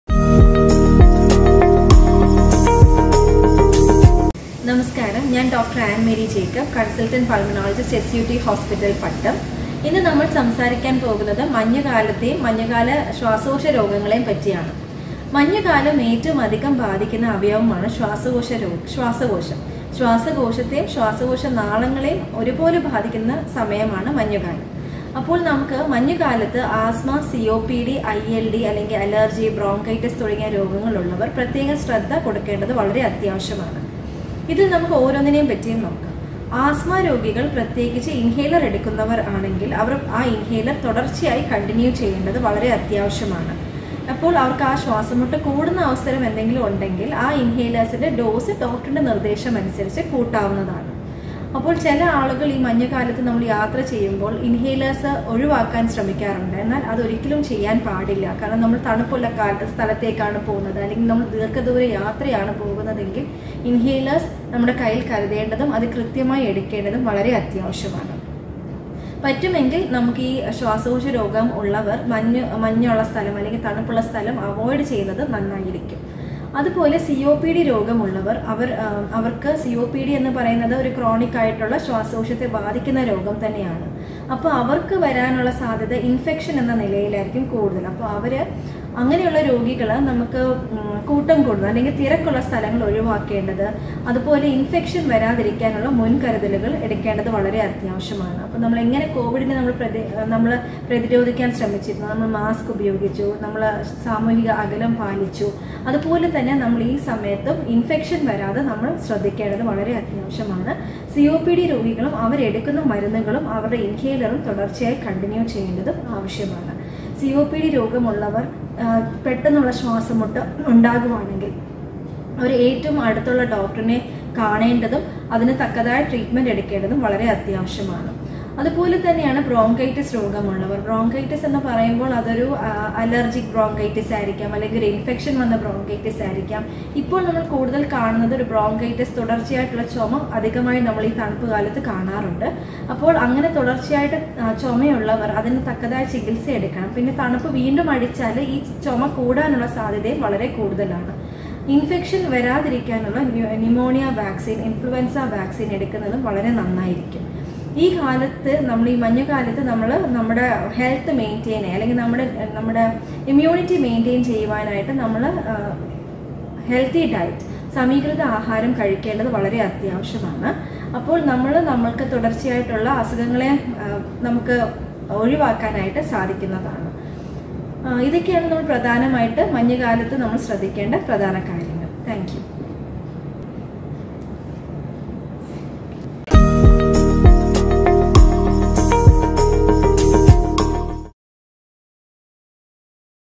ശ്വാസകോശ രോഗങ്ങൾ എന്തെങ്കിലും ഉള്ളവർ മഞ്ഞു കാലത്ത്‌ ശ്രദ്ധിക്കേണ്ട കാര്യങ്ങൾ, SUT Ep 340
ശ്വാസകോശ രോഗങ്ങൾ എന്തെങ്കിലും ഉള്ളവർ മഞ്ഞു കാലത്ത്‌ ശ്രദ്ധിക്കേണ്ട കാര്യങ്ങൾ | Dr. Ann Mary Jacob, Pulmonologist, SUT Hospital Pattom Trivandrum, BRLife | SUT Hospital Health & Wellness | Episode 340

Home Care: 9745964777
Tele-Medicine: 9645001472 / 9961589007
Booking Landline Number: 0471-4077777 / 4077888